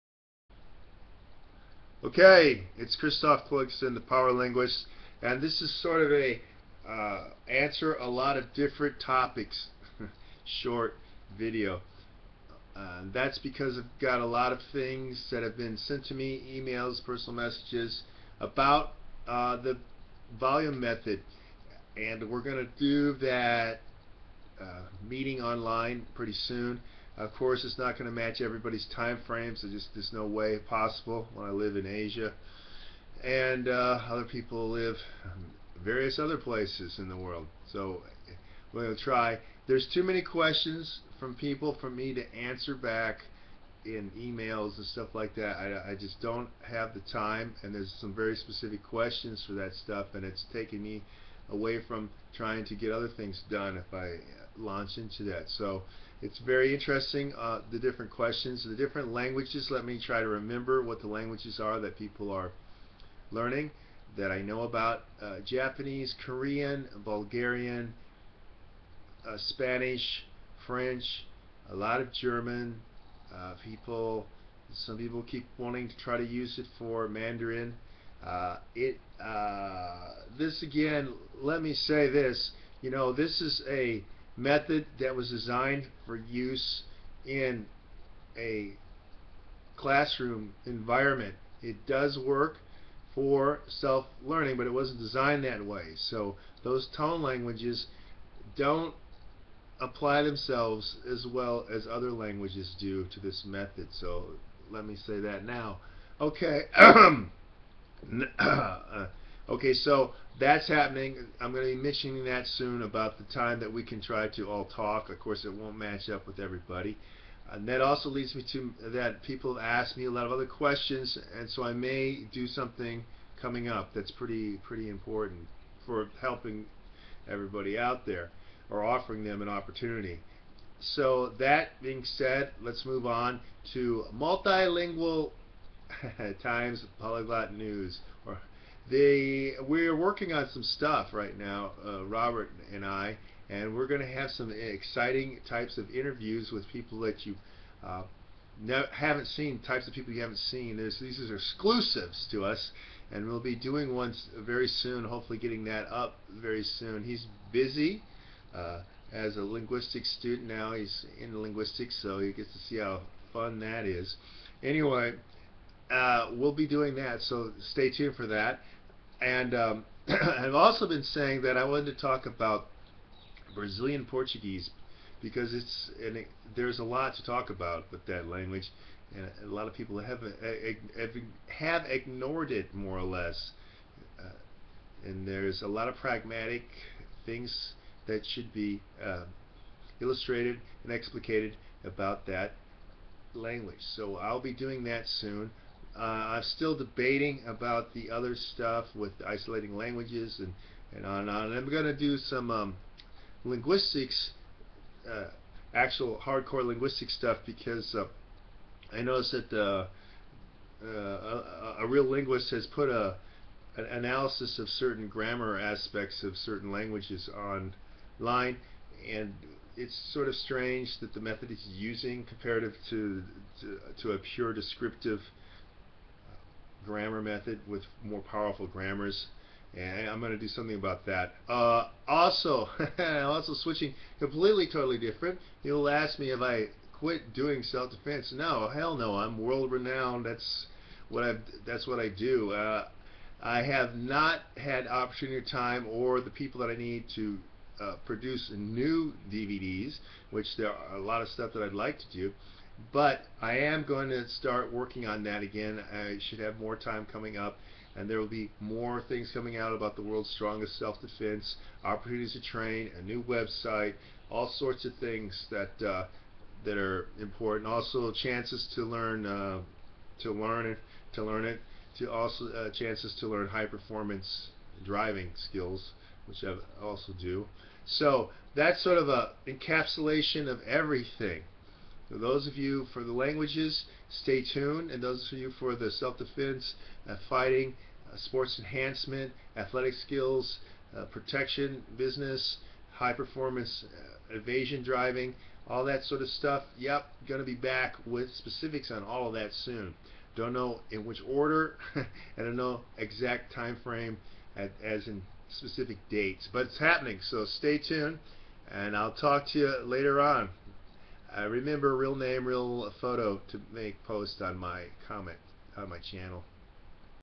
You asked questions: self defense, languages, driving, Christophe Clugston
This covers a lot of ground in languages, fighting, self defense, performance driving. So give it a listen, from the Power Linguist, bad boy of linguistics, Christophe Clugston